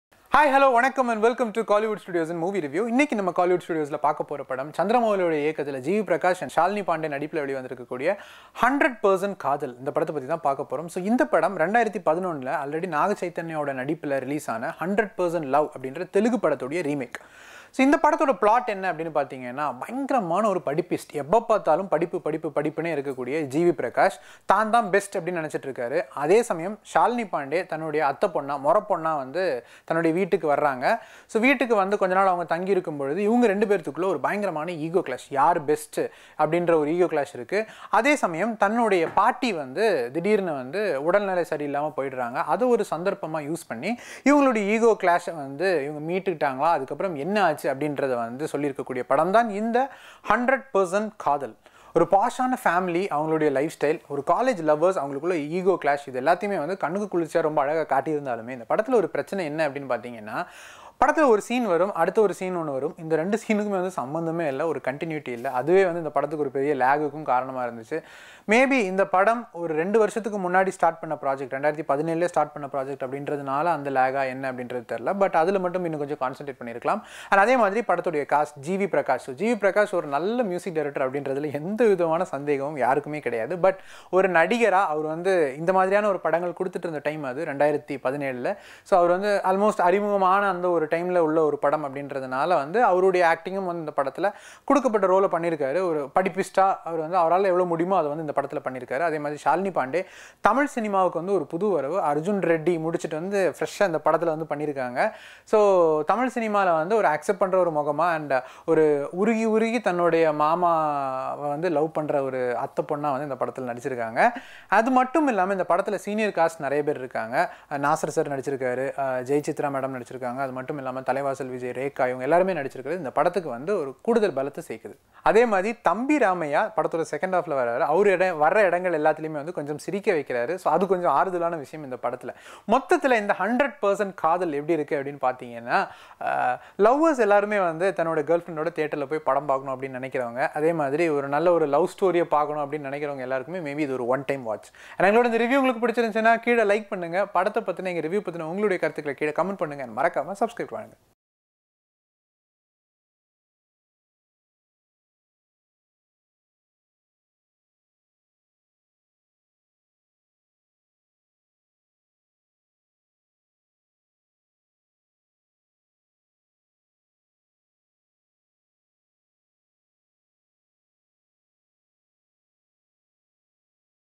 100% Kadhal Movie Review | GV Prakash | Shalini Pandey | Jaya TV
#100%Kadhal is a 2019 Indian Tamil-language romantic comedy film directed by Chandramouli and starring G.V.Prakash Kumar and ShaliniPandey in the leading roles. A remake of the Telugu film 100% Love, the film began production in October 2017. The film had its thetrical release on 4 October 2019.

Category Entertainment